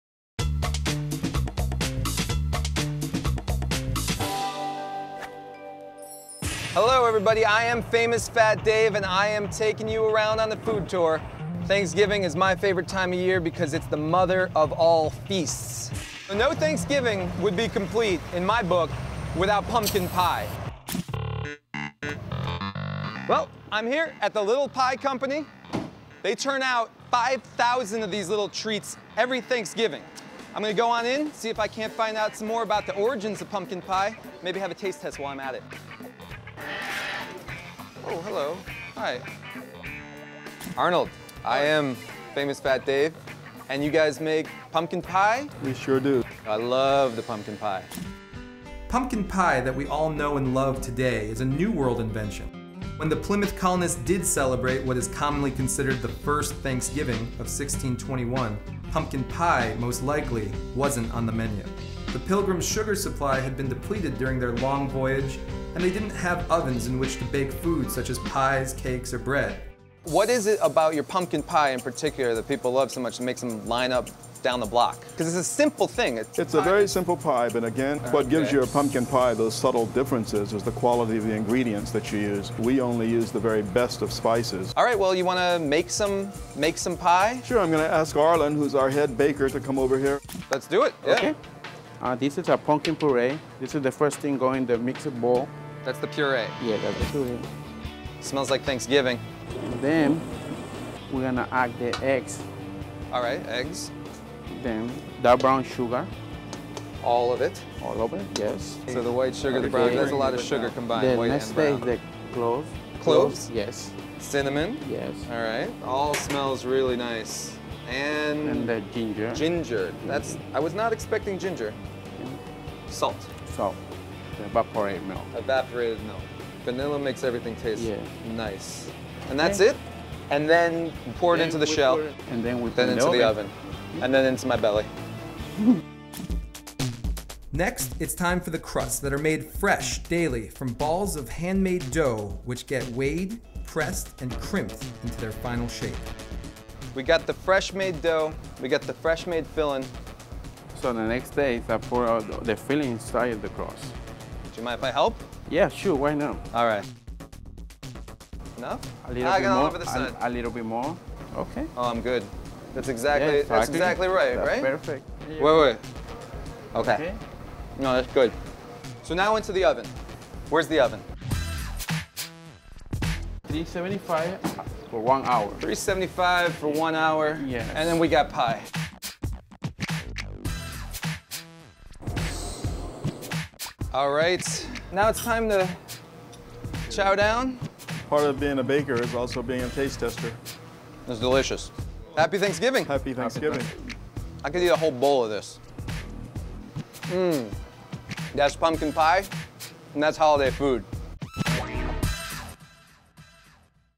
Famous Fat Dave: Thanksgiving Pumpkin Pie
Dave learns to make pumpkin pie at The Little Pie Company.